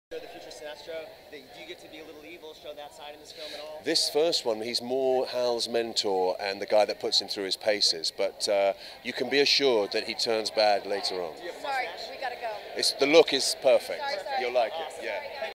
Green Lantern - Comic-Con 2010 Exclusive: Mark Strong
We caught up with actor Mark Strong to talk about his role in the Green Lantern.